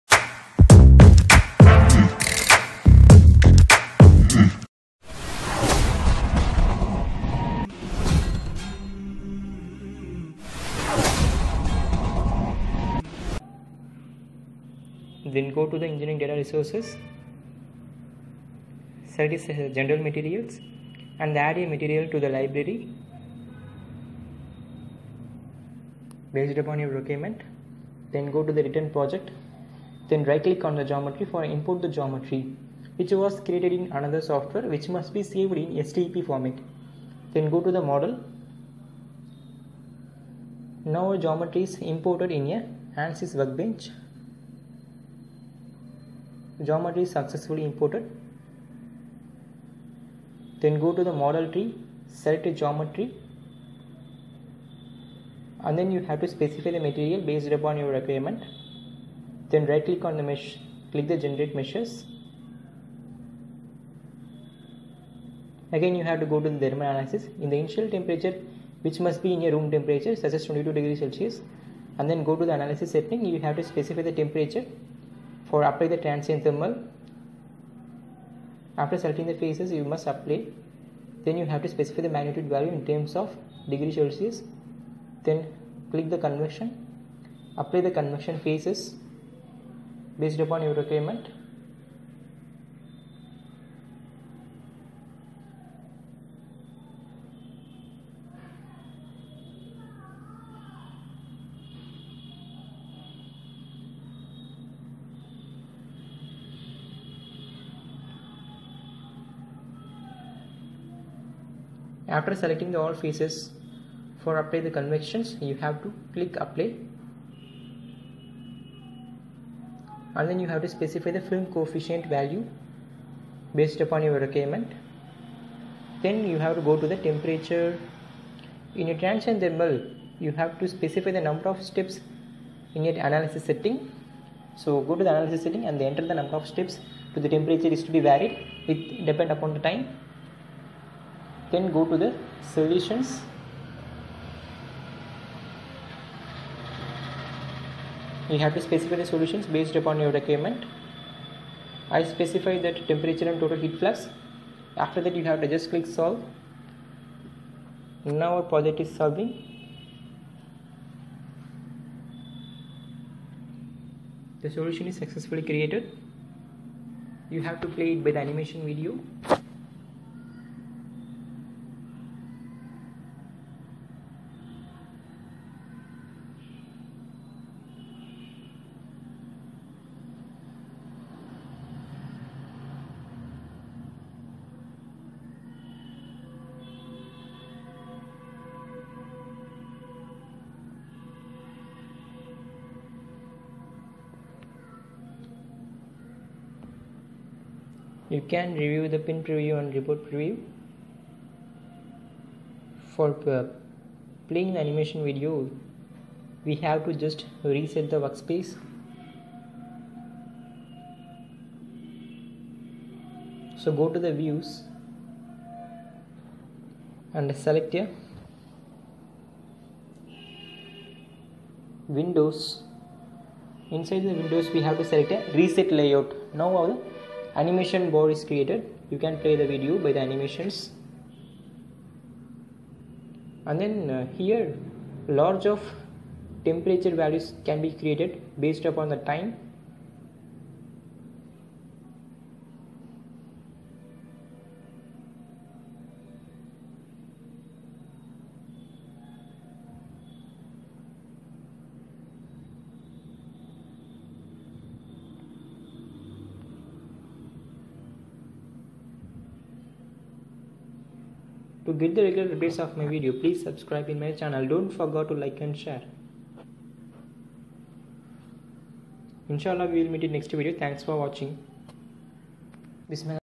TRANSIENT THERMAL ANALYSIS OF PISTON IN ANSYS WORKBENCH
✓stress analysis of piston using pressure ✓load and thermal load